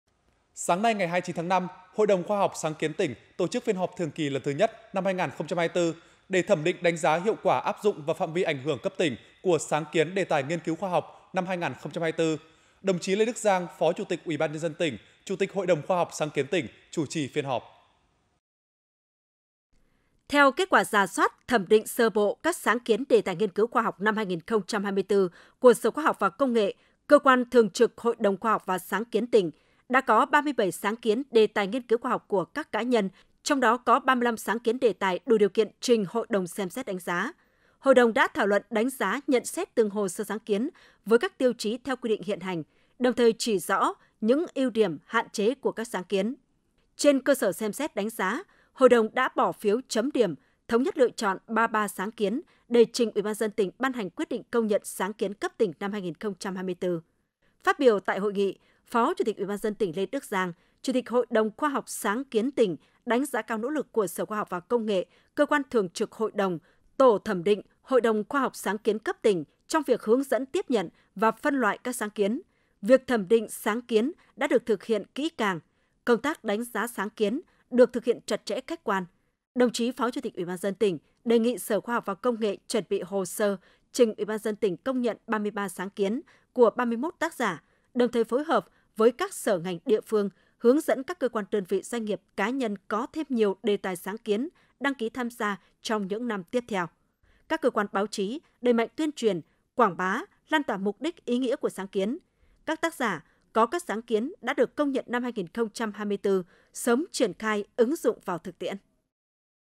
Hội đồng Khoa học và sáng kiến tỉnh Thanh Hóa họp thường kỳ
Sáng ngày 29/5, Hội đồng Khoa học, sáng kiến tỉnh tổ chức phiên họp thường kỳ lần thứ nhất, năm 2024 để thẩm định, đánh giá hiệu quả áp dụng và phạm vi ảnh hưởng cấp tỉnh của sáng kiến, đề tài nghiên cứu khoa học năm 2024. Đồng chí Lê Đức Giang, Phó Chủ tịch UBND tỉnh, Chủ tịch Hội đồng Khoa học, sáng kiến tỉnh chủ trì phiên họp.